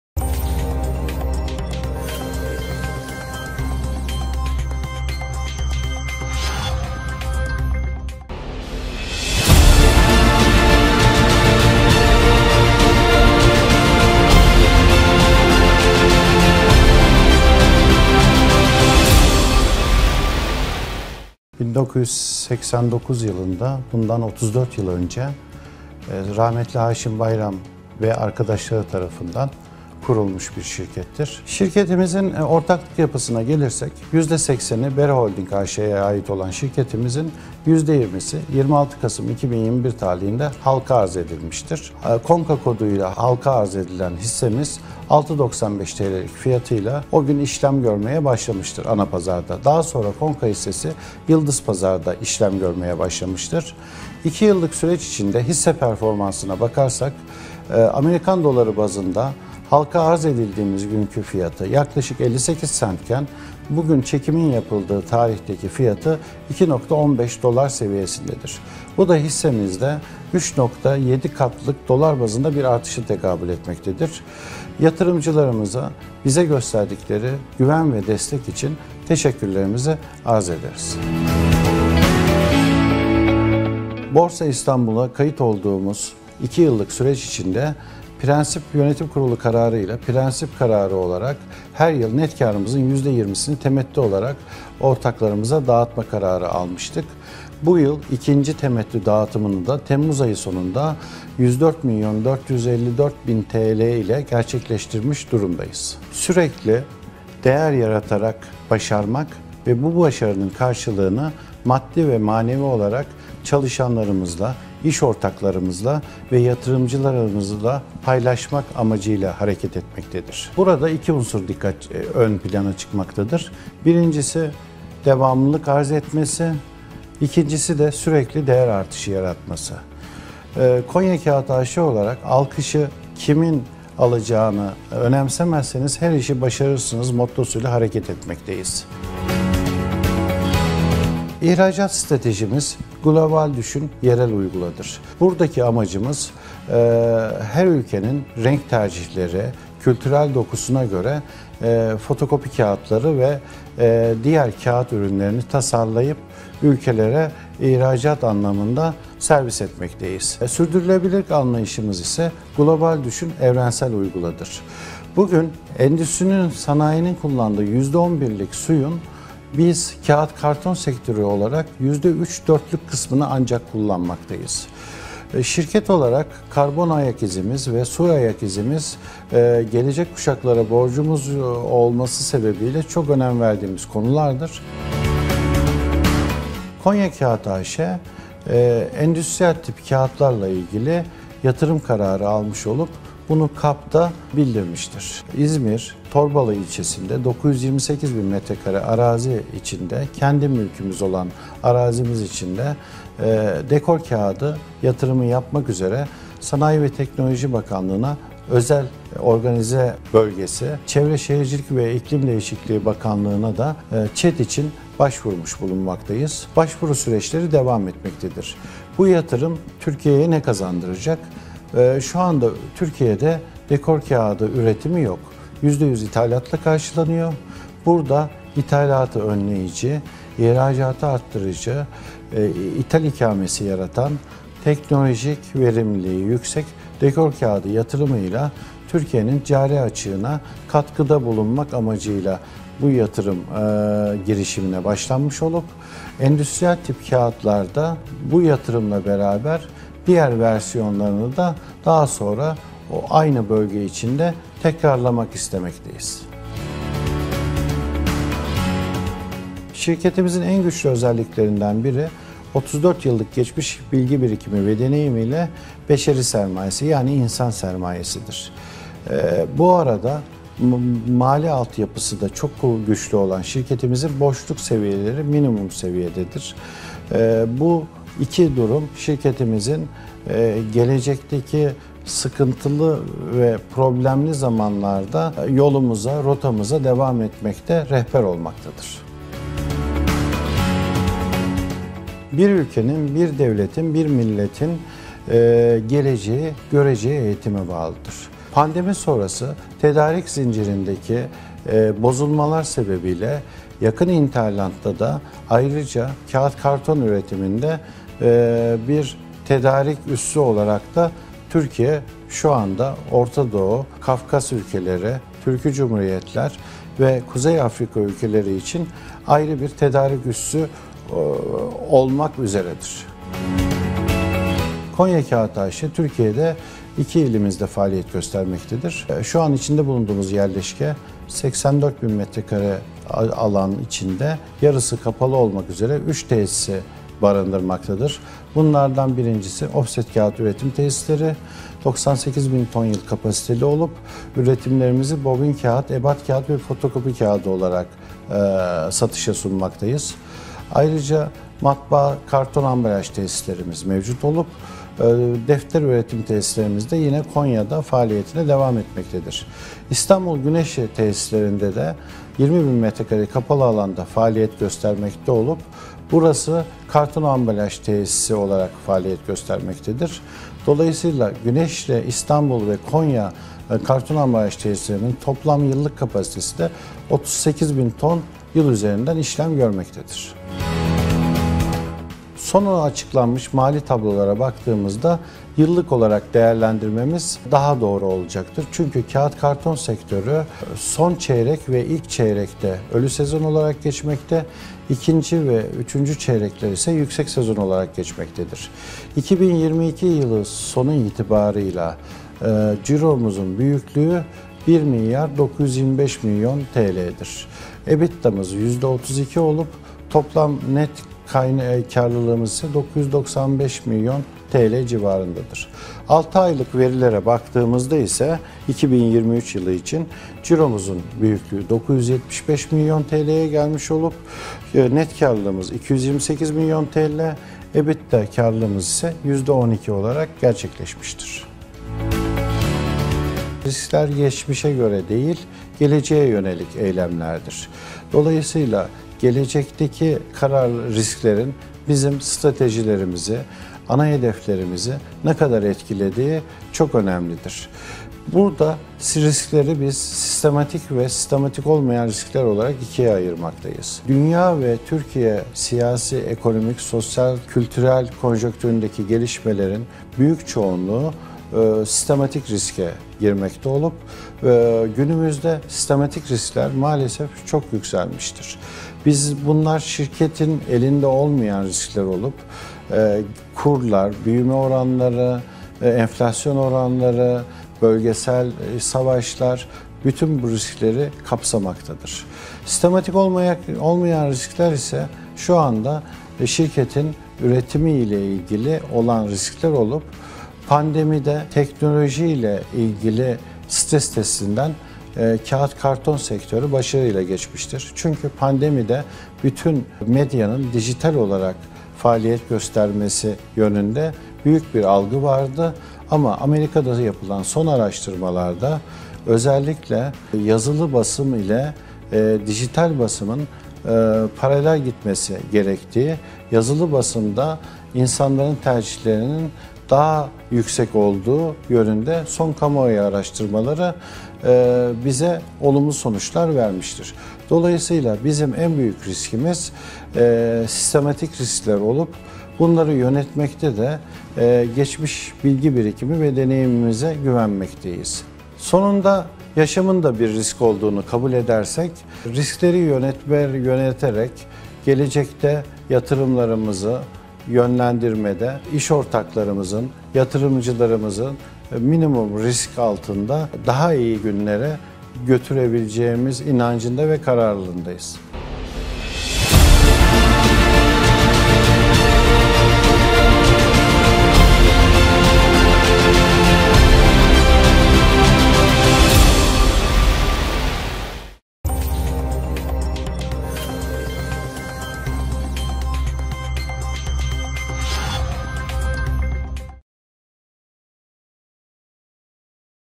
"Borsa İstanbul Kote Şirketlerini Tanıtıyor!"
"Borsa İstanbul Kote Şirketlerini Tanıtıyor!" projesi kapsamında iştirakimiz olan Konya Kağıt A.Ş tanıtım filmi Borsa İstanbul tarafından yayınlanmıştır.🎥